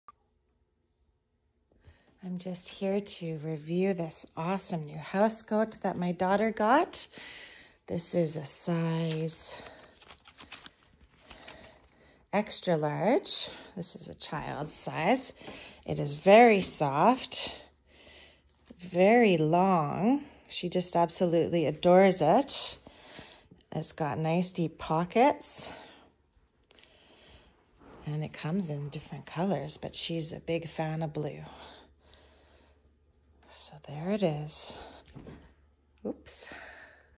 Review- TowelSelections Kids Soft Fleece Robe Housecoat
So soft and warm! Here is my review for my daughter's perfect bathrobe.
It was the perfect gift for her!